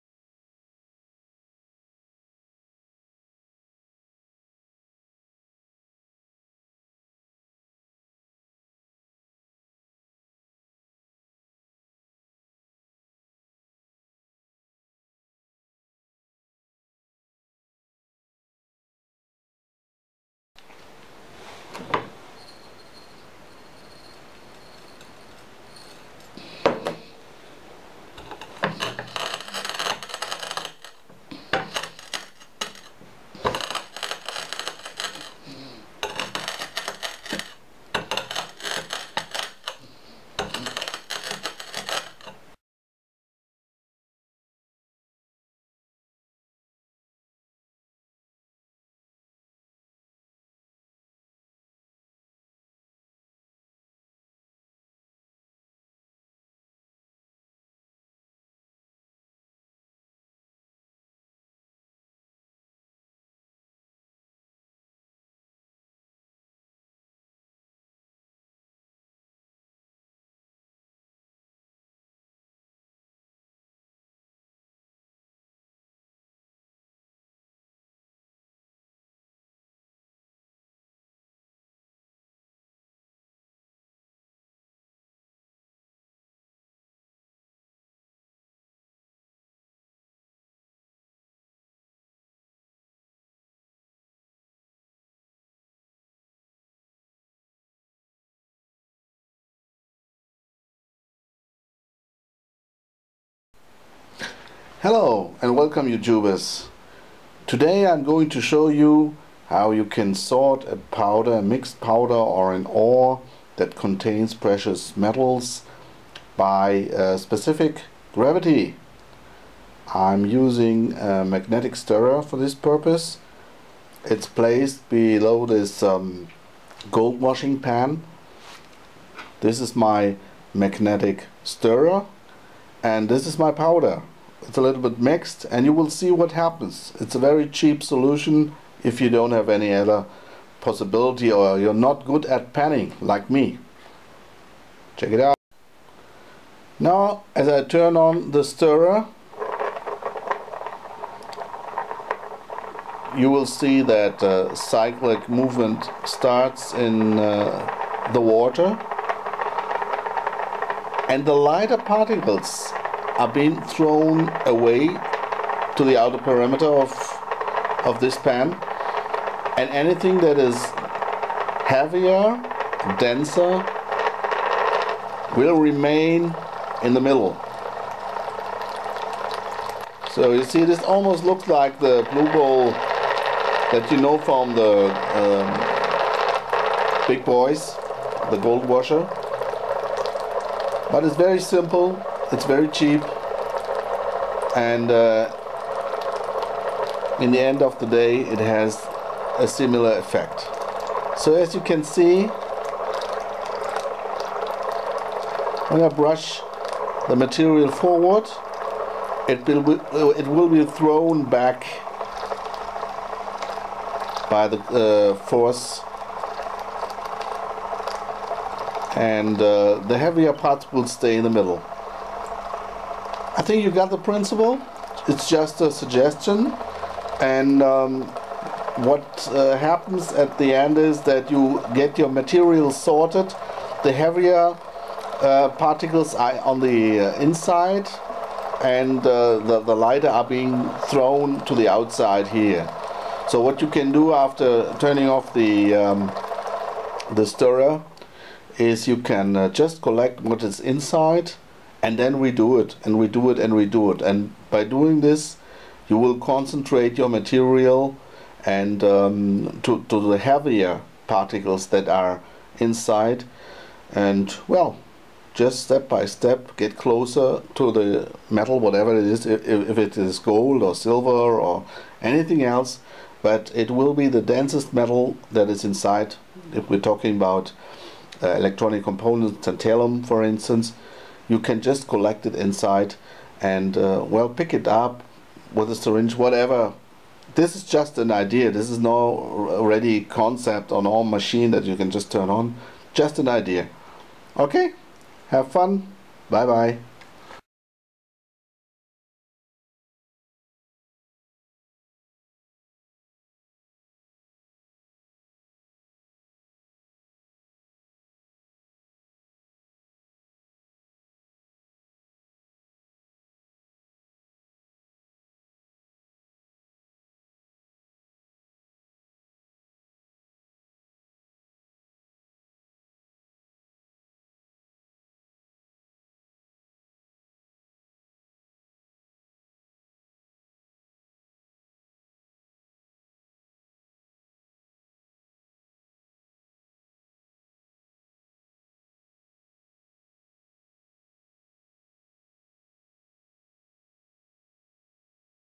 How to extract gold from electronic scrap
Small scale gold silver palladium extracting project with results